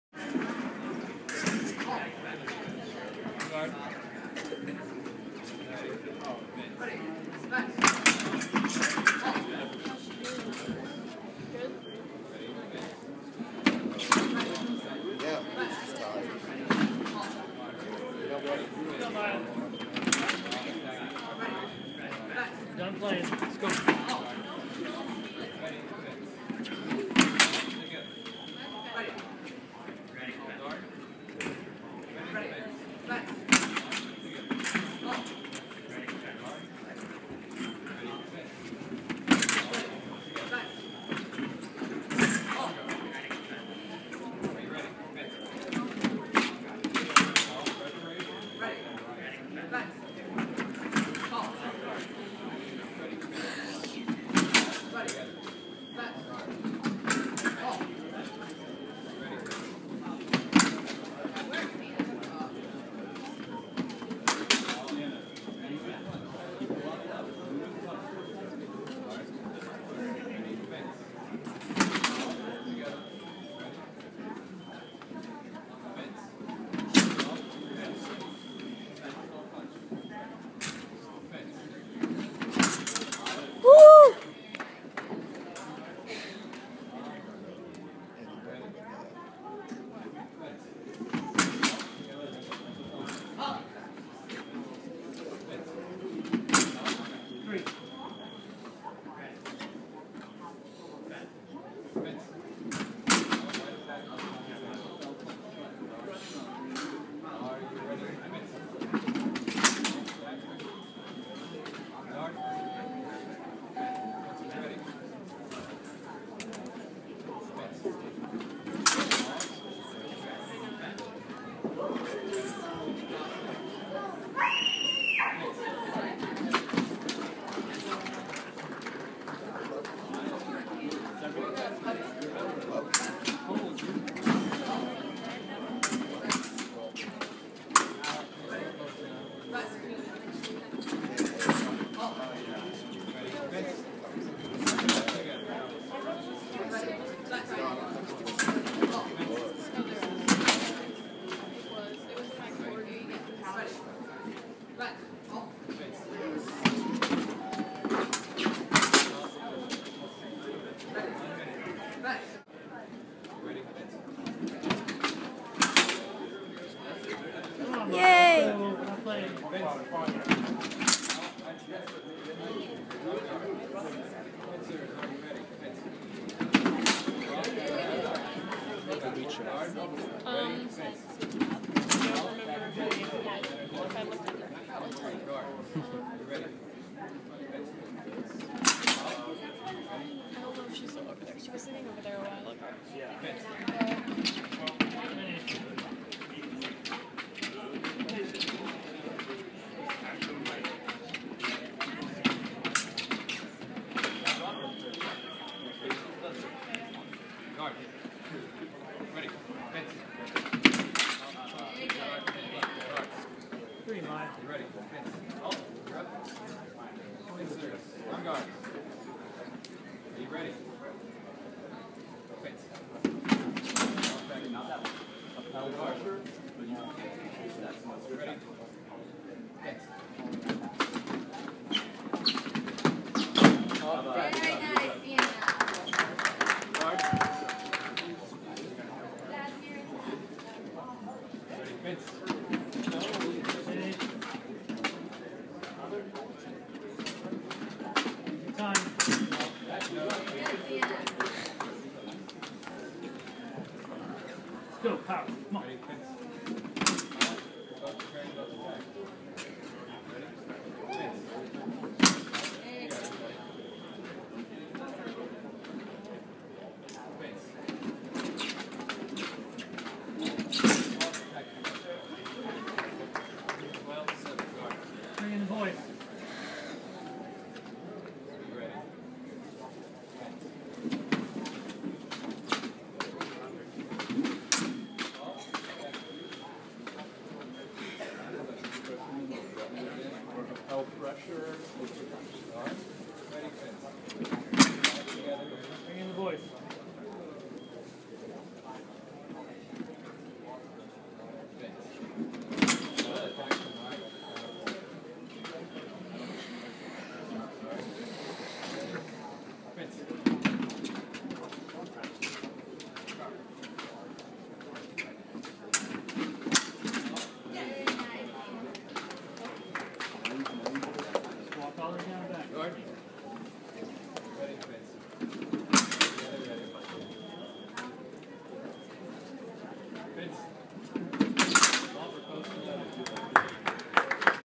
Arnold Fencing Classic 2014 Szonn-Lillard vs Sienna Wilson
Arnold Fencing Classic 2014
Y14 Women's Saber
Maya Szonn-Lillard (Columbia Fencing and Fitness) vs Sienna Wilson (Cheney Fencing)
Final Score: 15_8 Szonn-Lillard